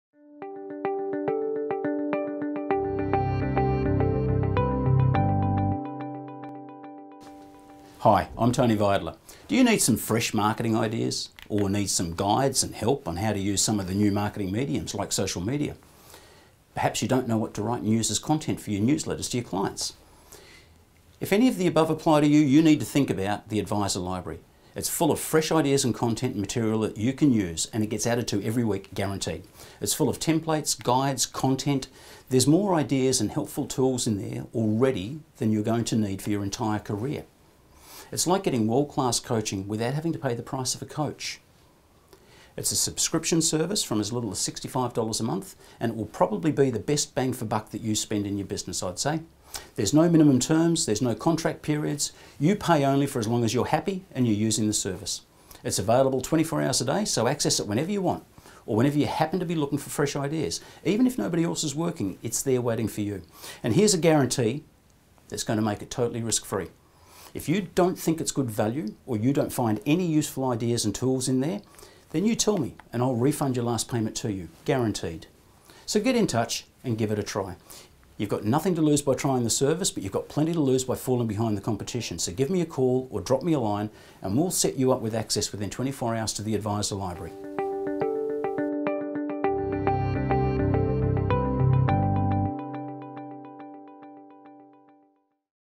Adviser Library Introduction
If you are looking for ideas, templates, guides, and ready to go business tools then the Adviser Library is well worth looking at.  In this quick video Tony Vidler discusses how it works - throws in a clear guarantee - and provides an overview of what to expect.